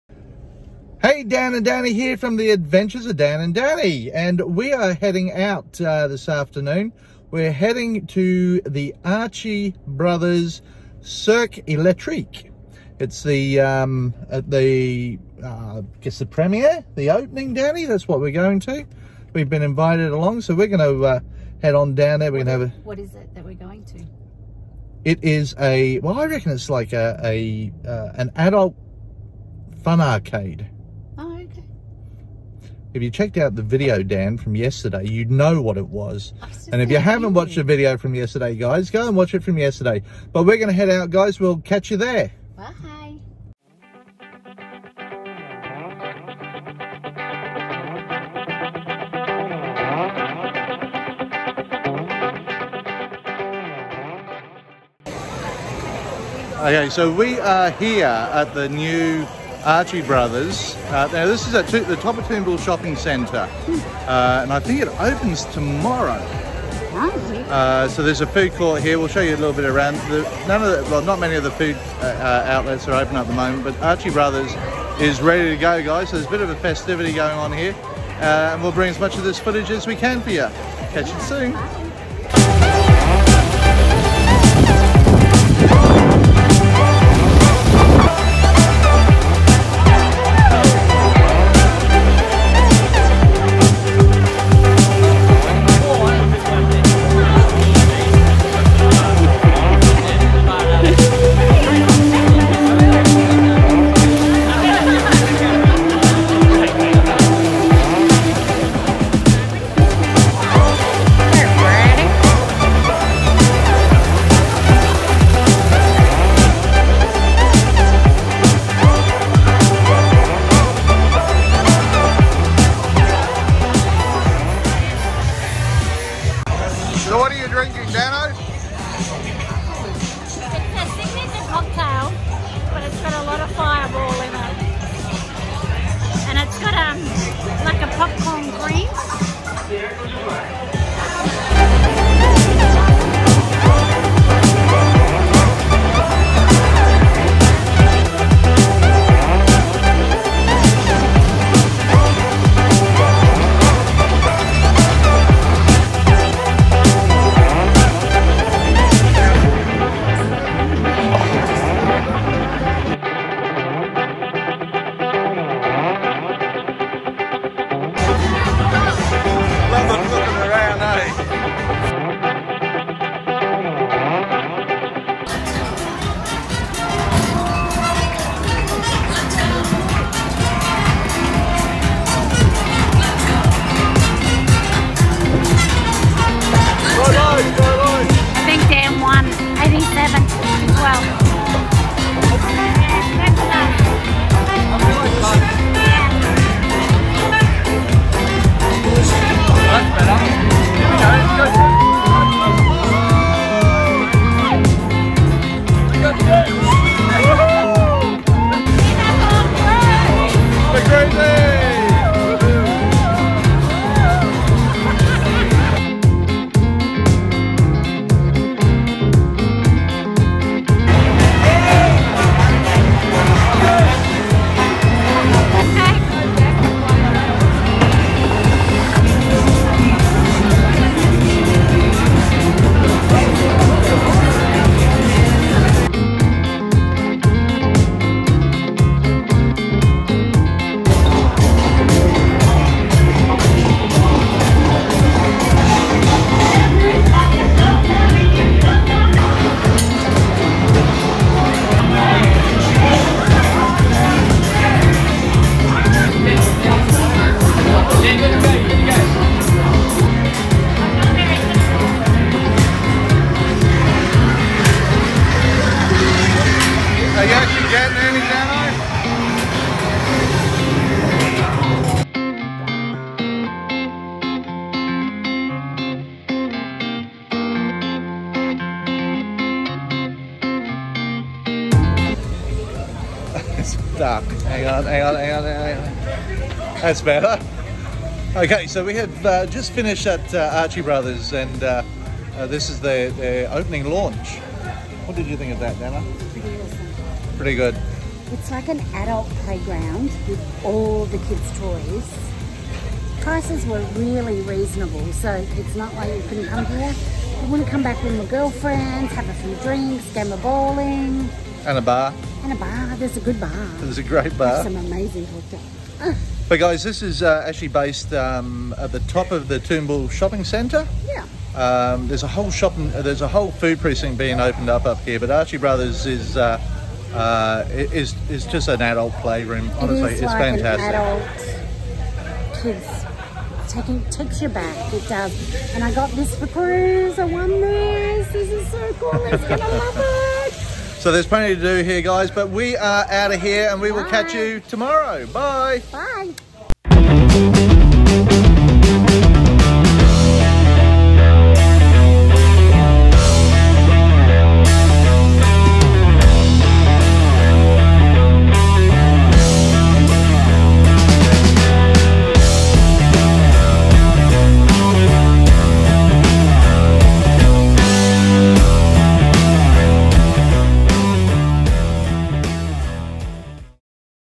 15th Nov Vlog, Archie Brothers Launch Party.
Dan and Dannii are invited to the Launch Party of Archie Brothers Newest Venue in Toombul Shopping Centre  Queensland.